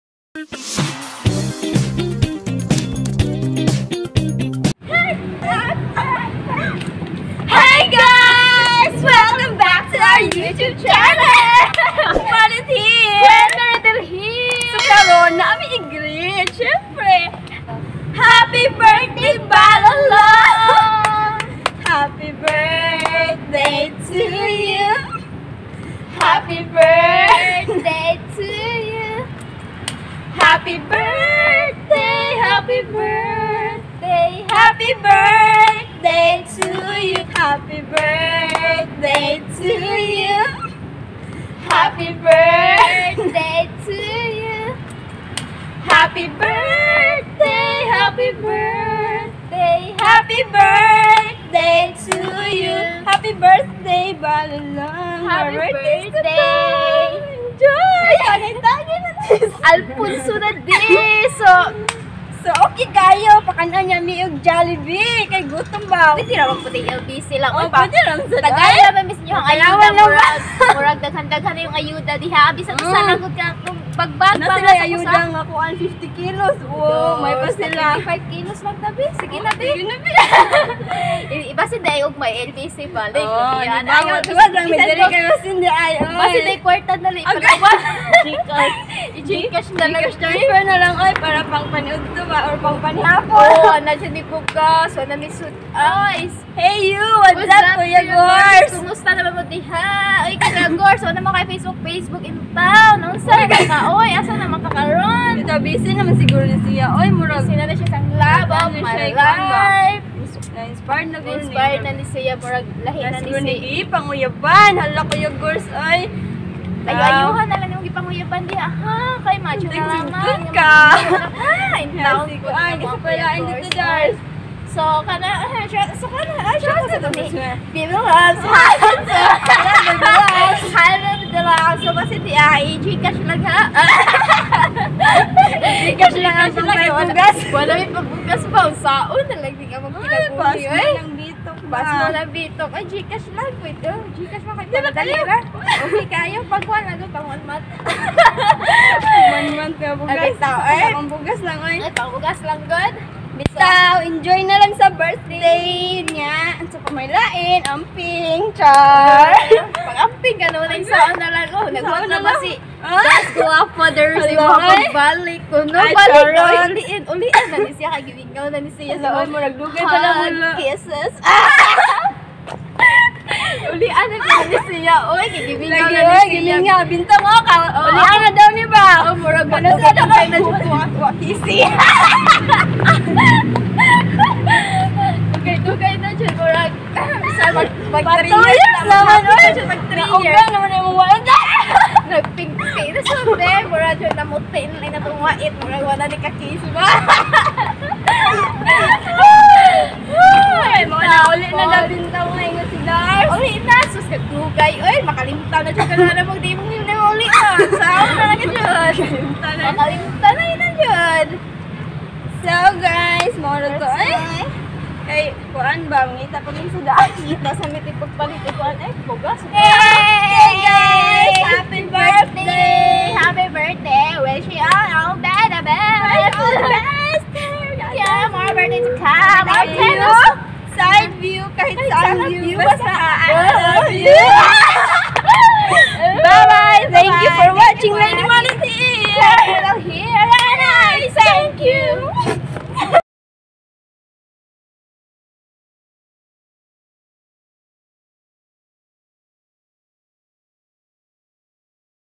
HAPPY 26TH BIRTHDAY BALOLONG 😍
Heyy wazzap Balolong 🥰

Kahibaw ka??
Nasakit ko tungod sa way klarong panahon,
Panahon nga .
wala ka sa akong tapad karon 😆

👉 LDR means.
L - Layo sa akong mga kamot
D - Duol sa akong dughan
R - Ready maghuwat 💜

Laban lang haha see you when I see you again😆 Amping diha , Lablab 🥰

Love ,
Lady Marlet Vlogs